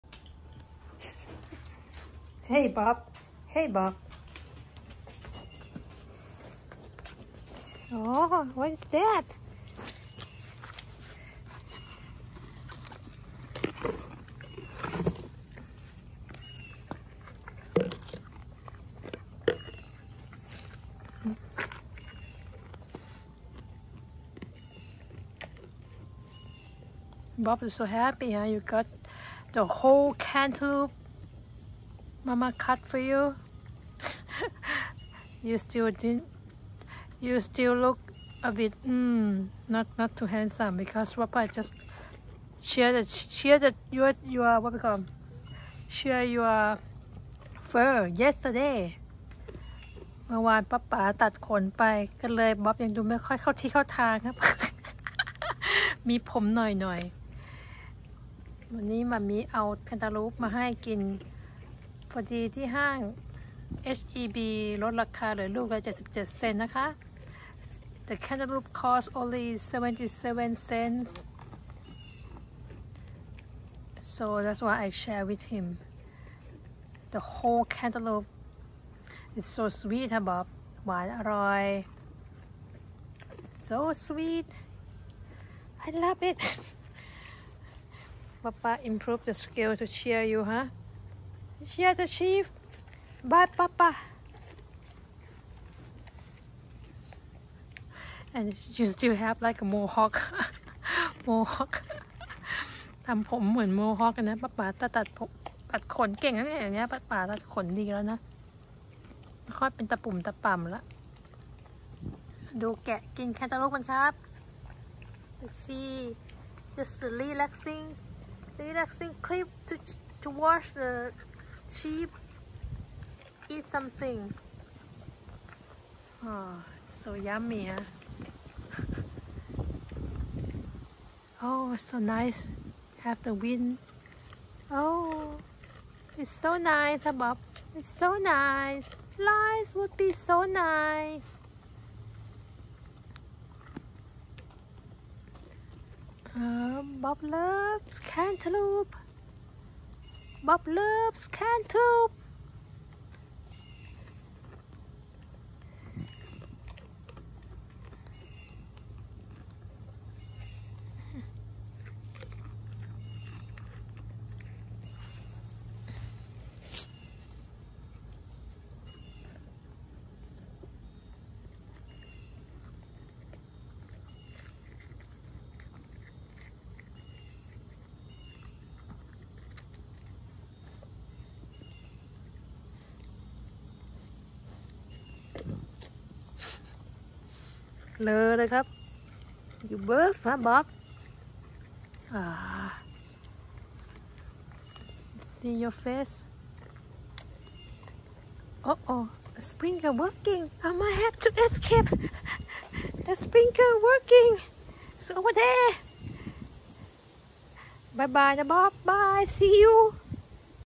Th/Eng: Bob and Cantaloupe เจ้าแกะบ๊อบกับแคนตาลูป
วันนี้กลับจากห้างใกล้บ้านแล้วได้แคนตาลูปหวานอร่อยราคา 77 เซนต์เลยซื้อมาฝากบ๊อบด้วย ได้กินเกือบทั้งลูกไปเลย แม่ต่ายมีหั่นไปขิมไปเพราะมันหวานอร่อยมากๆ

Today I got 77 cents cantaloupe from H‑E‑B store so I cut it for Bob my sheep. It’s made his day as he really loved eating it so much. I knew it’s so sweet as I tasted some pieces while cut for him.